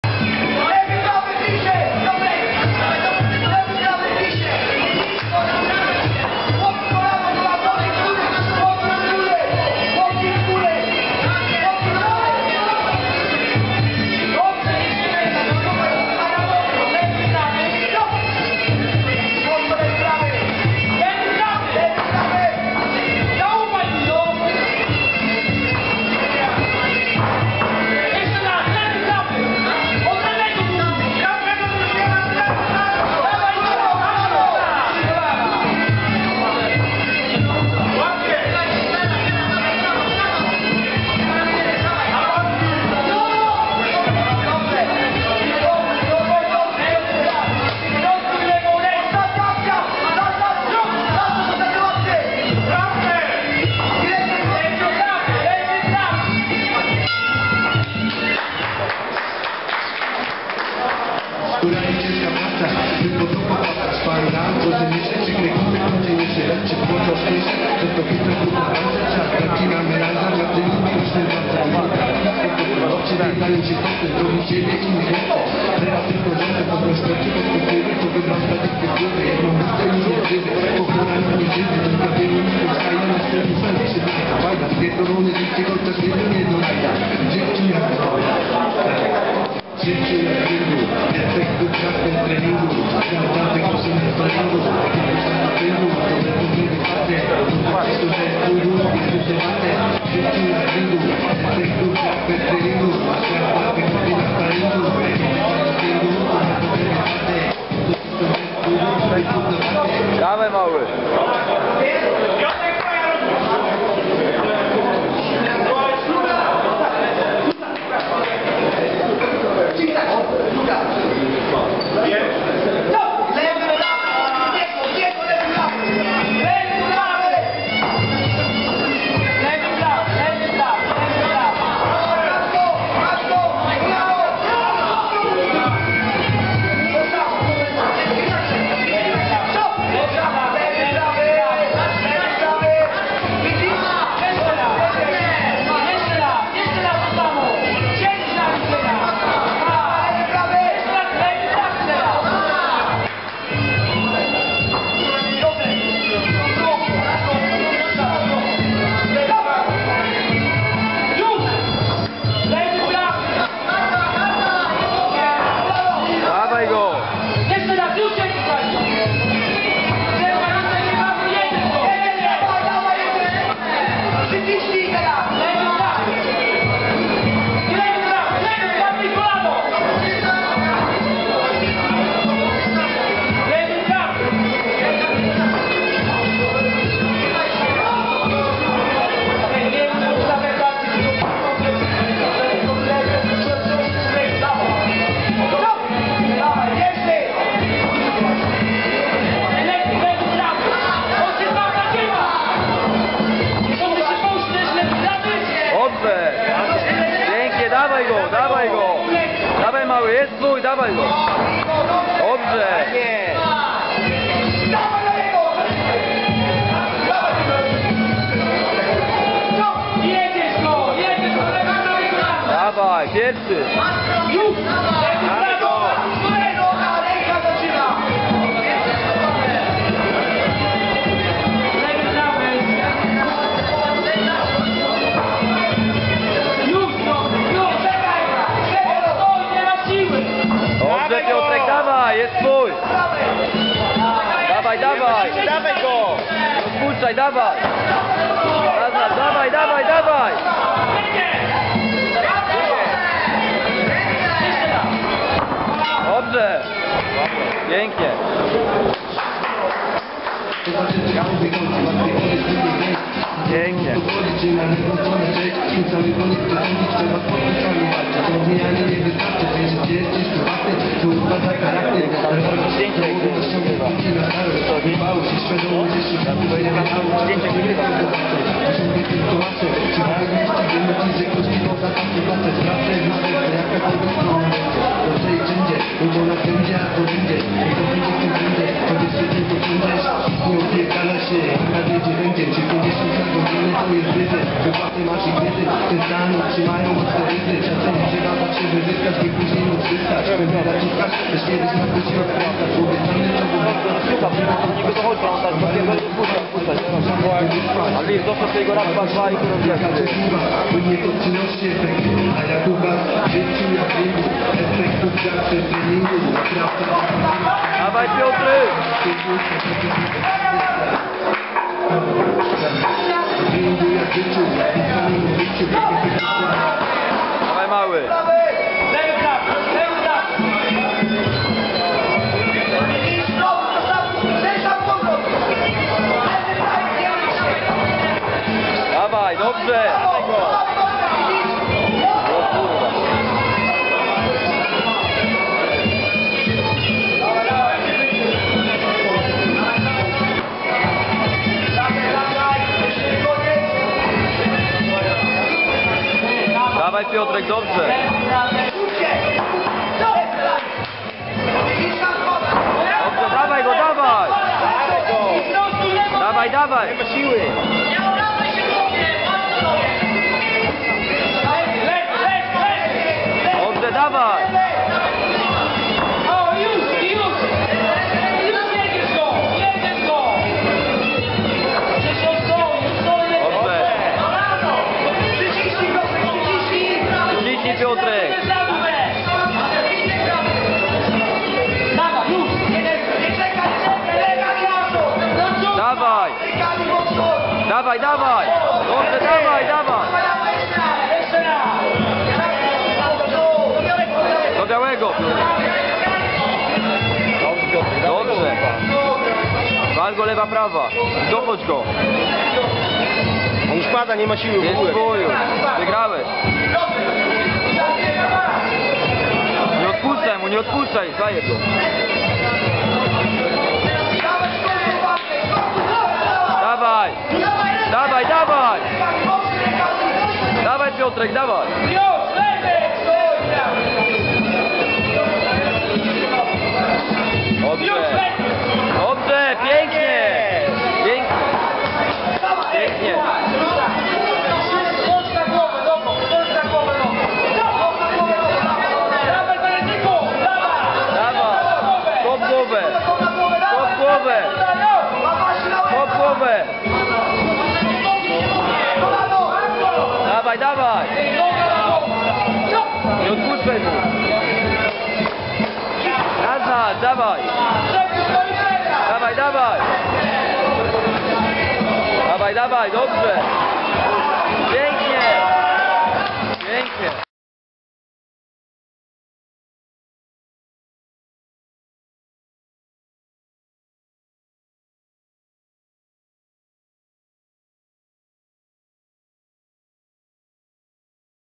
Piotr Markowski - Paweł Łukawiecki Summer Cup Muai Thai 30.06.2012 Kraków
Piotr Markowski ExtremeFighters Wrocław kask czarny vs. Paweł Łukawiecki Rio Grappling Ttychy kask niebieski.